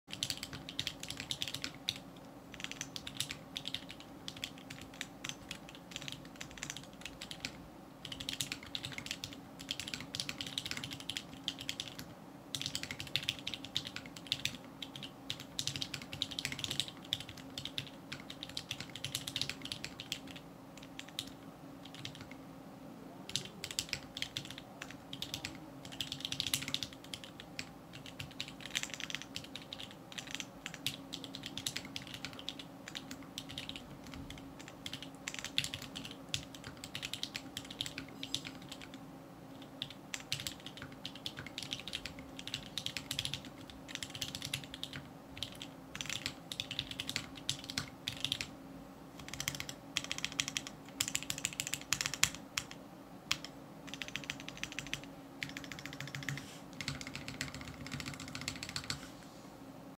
Type Test KPRepublic 60% & Outemu Sky 68g
KPRepublic 60% w/ Outemu Sky Switches Lubed w/ 3204 and clipped/bandaid modded stabilizers